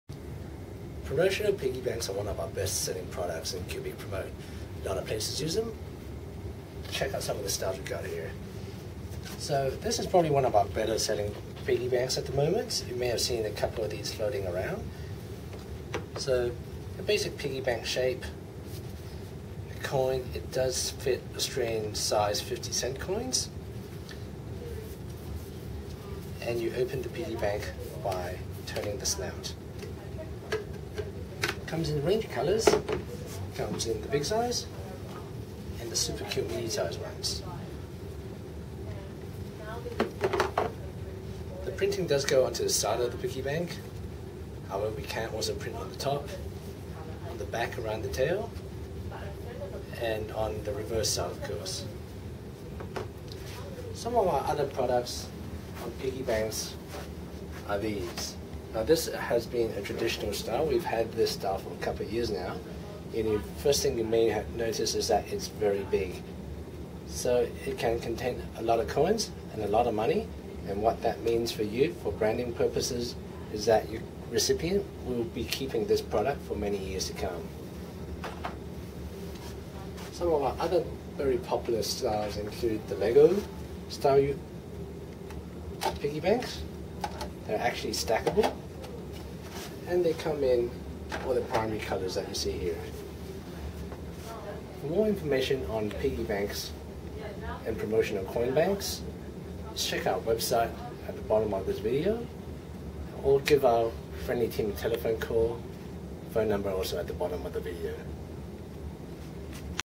Promotional Piggy Banks
Custom branded piggy banks with your logo are a hot selling promotional product.  They are not just for banks either.  Use them in tradeshows or as fund raisers.  They will be a definite hit.